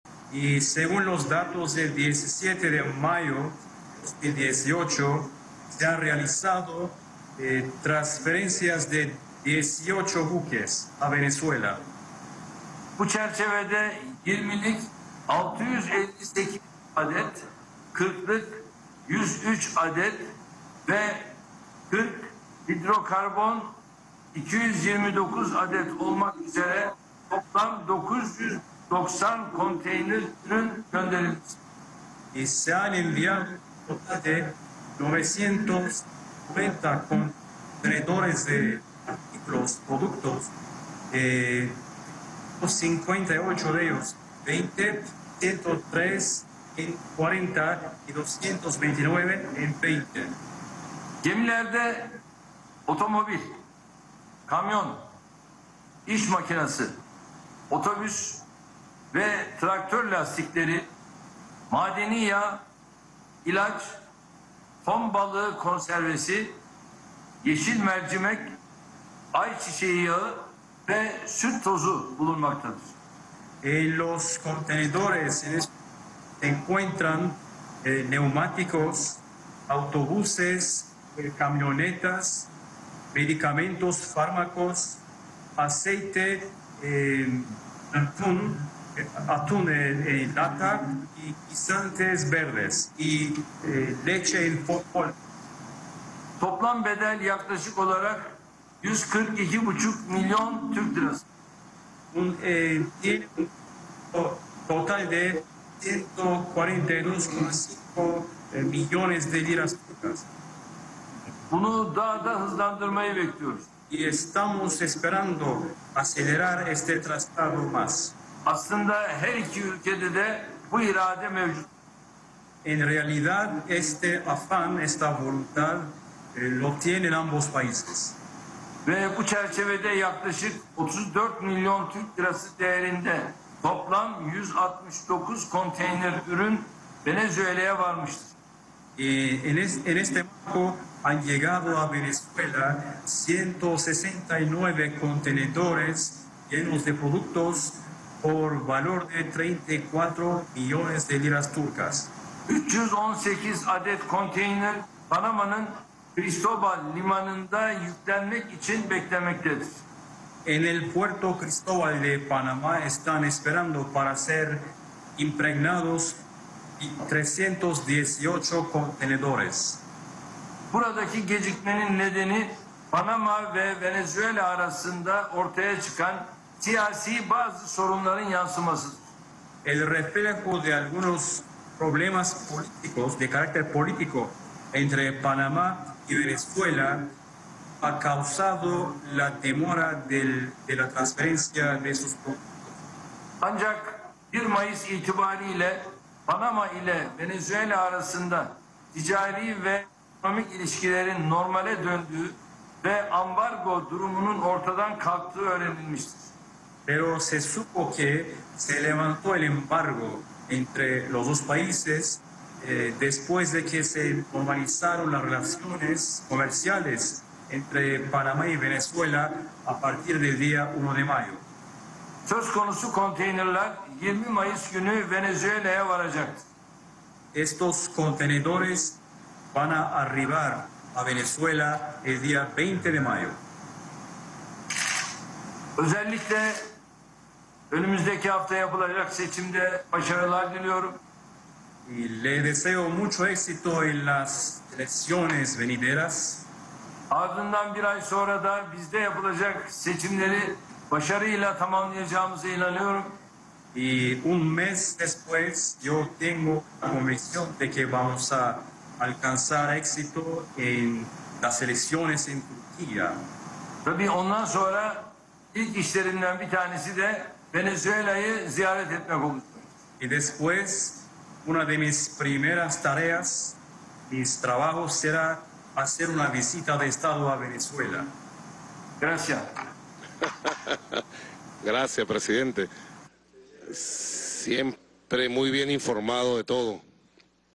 Erdogan, Presidente de Turquía: El 20 de mayo llegarán a Venezuela 318 containers desde Turquía
Recep Tayyip Erdogan, presidente de Turquía, afirmó en videoconferencia con su homólogo, el venezolano Nicolás Maduro, que  el próximo 20 de mayo arribarán a Venezuela más de 318 contenedores con alimentos, neumáticos y medicinas, que no habían podido llegar al país por estar retenidos en Panamá.